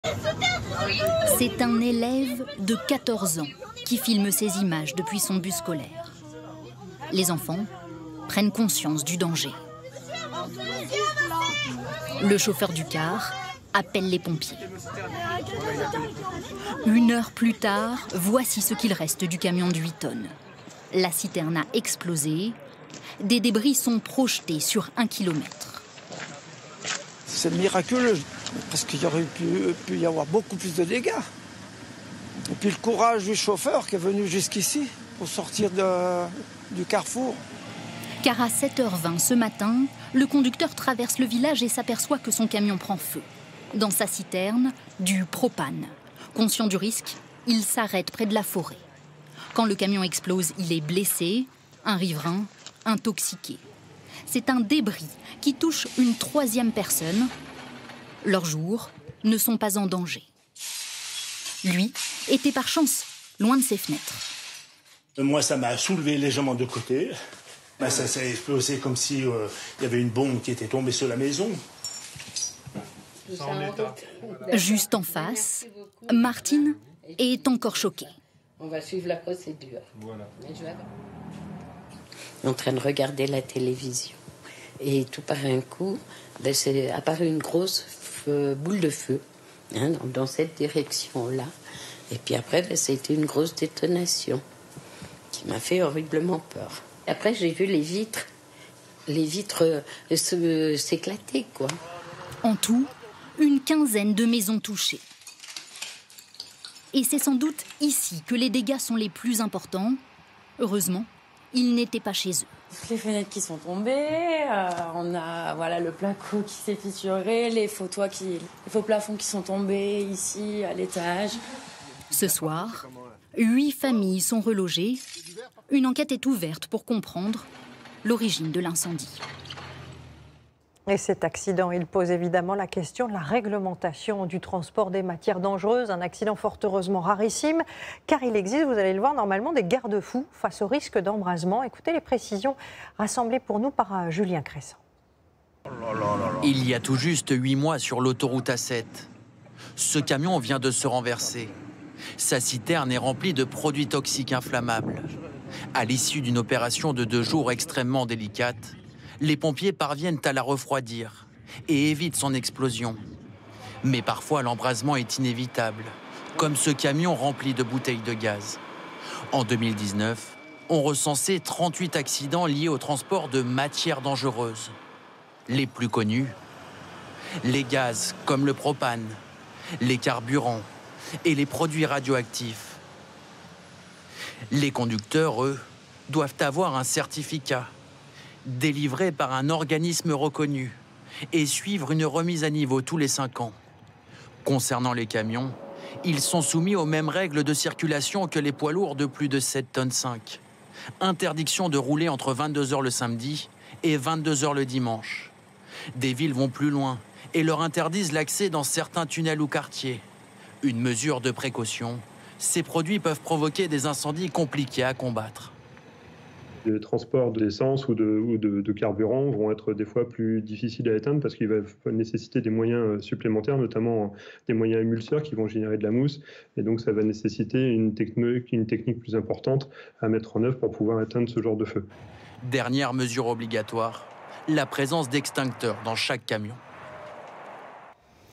Explosion d'un camion-citerne : des dégâts à 1km à la ronde
Un camion transportant du gaz a explosé vendredi matin à Fillinges (Haute-Savoie). Vingt-et-une personnes ont été impliquées dans l'accident, dont deux ont été blessées. Aux alentours, des dizaines de maisons ont été endommagées.